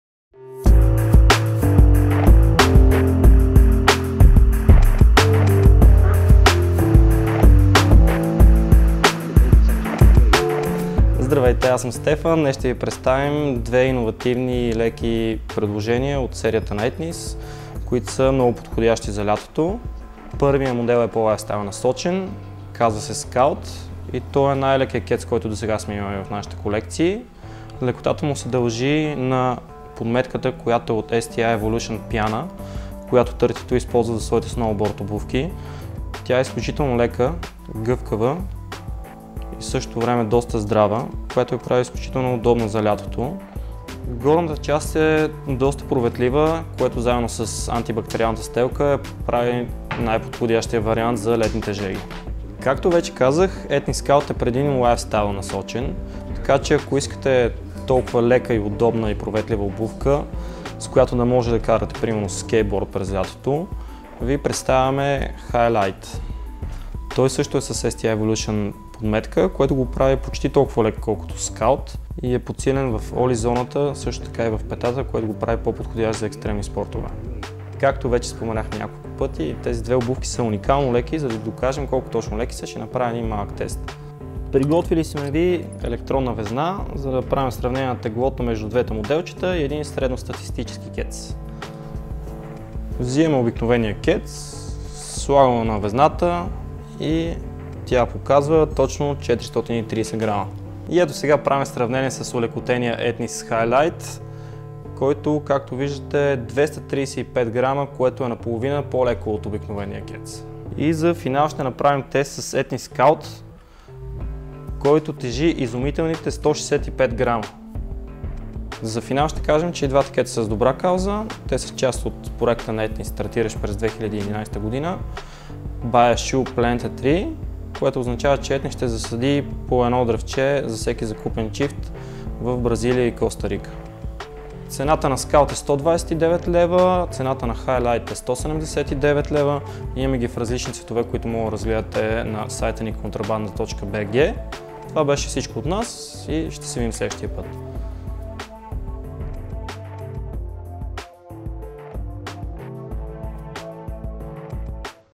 Contrabanda TV, Епизод 7 - Най-леките кецове на пазара
Епизод 7 е посветен на etnies Scout и etnies Highlight - два ултра леки, ултра здрави и ултра удобни модела за лятото. Единият е по-скоро за ходене, а другият е много подходящ и за каране на скейт, имайки предвид, че е про-модел на Тайлър Бледсоу.

Двата модела може да намерите, както в магазините ни, така и онлайн на